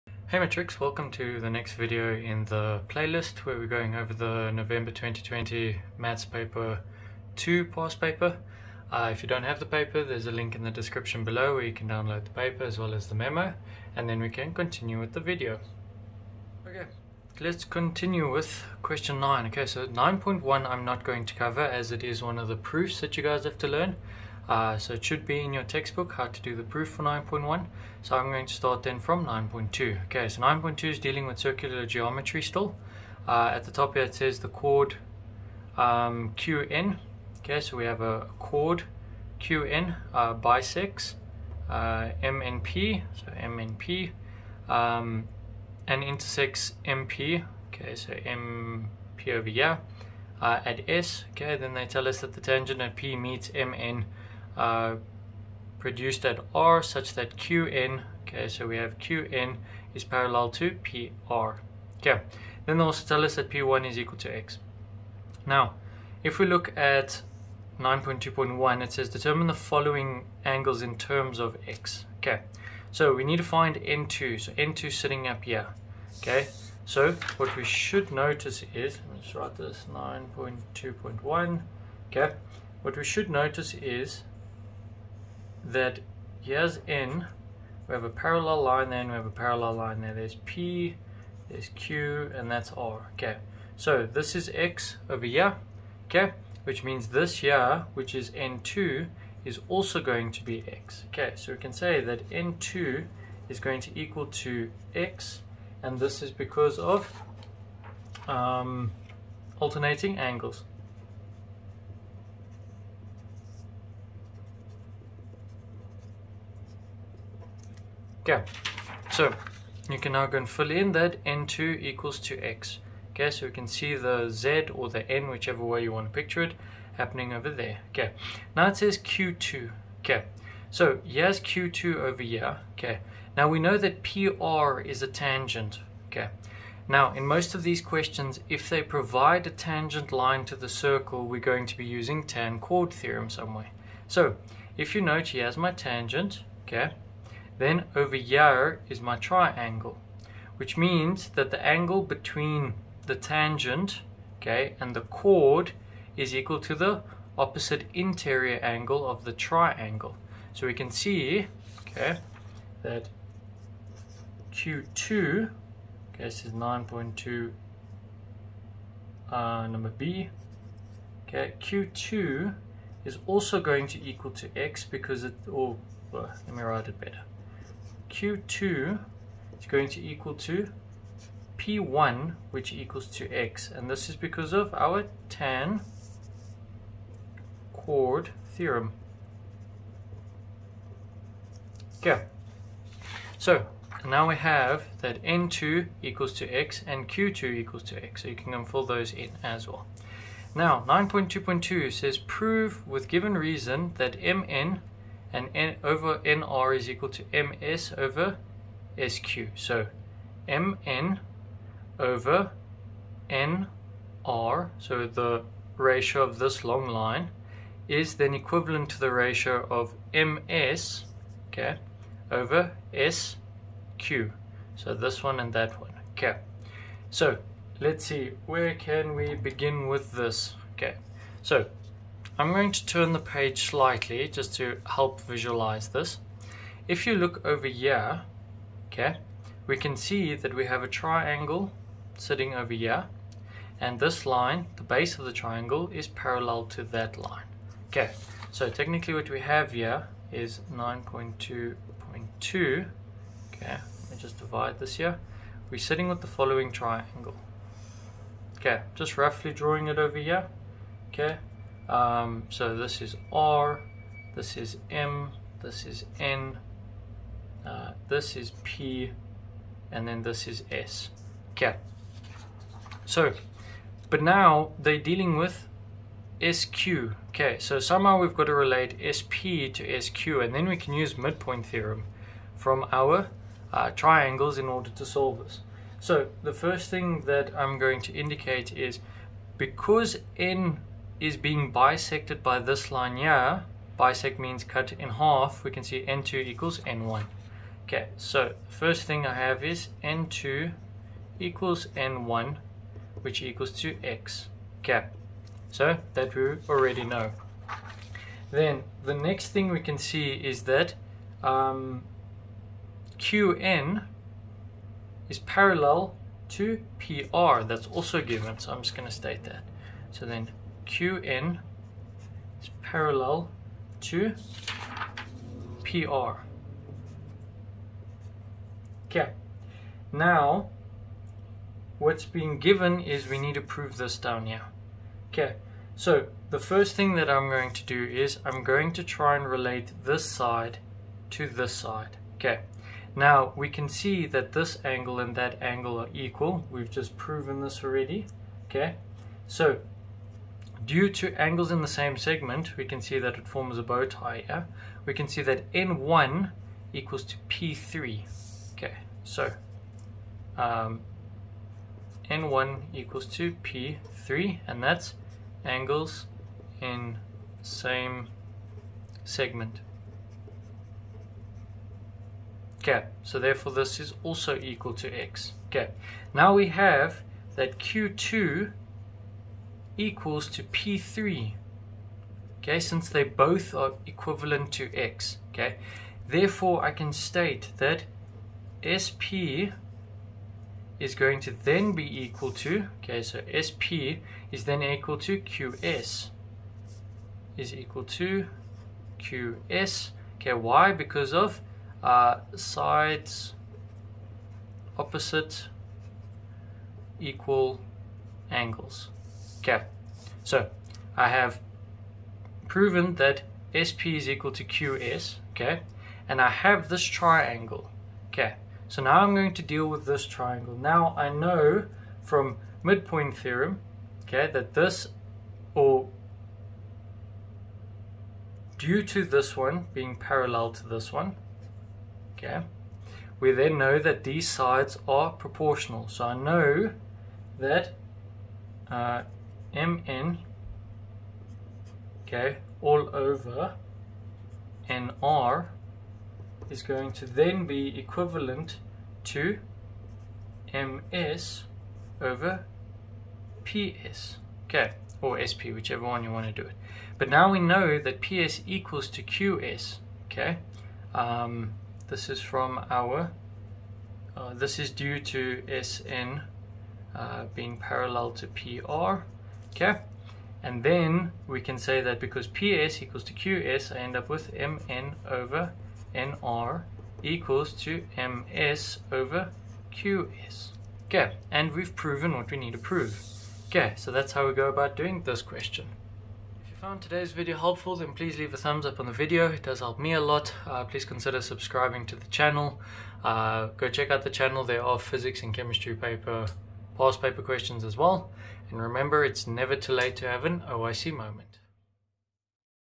Matric/Grade 12 Mathematics Past Paper 2 Question 10 Circular Geometry, congruency, proportionality
In this video I cove question 9 of the November 2020 math paper 2 past paper. The question contains a circular geometry proof that I do not cover. The second part of the question deals with circular geometry. You are asked to find the size of different angles in terms of x. Then you are asked to prove the proportionality theorem for a give triangle.